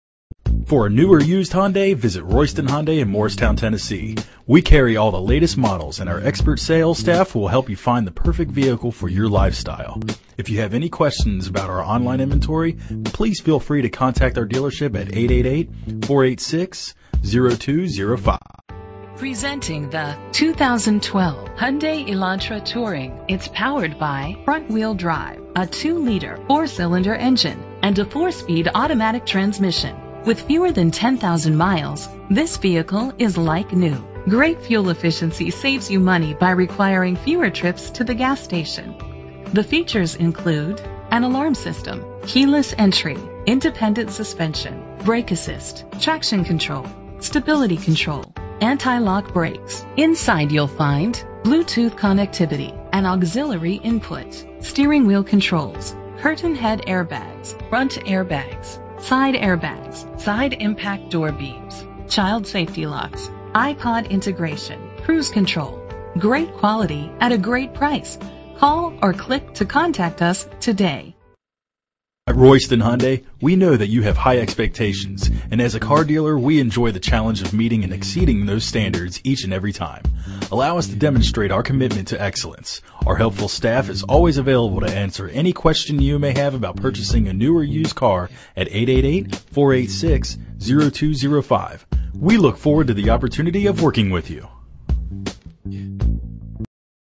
2012 Hyundai Elantra Touring 401136 - Morristown TN
Year: 2012
Make: Hyundai
Model: Elantra Touring
Trim: GLS
Engine: 2.0 liter 4 cylinder FWD
Transmission: 4-Speed Automatic
Color: White
Mileage: 9623
Address:
VIN:KMHDB8AE7CU151771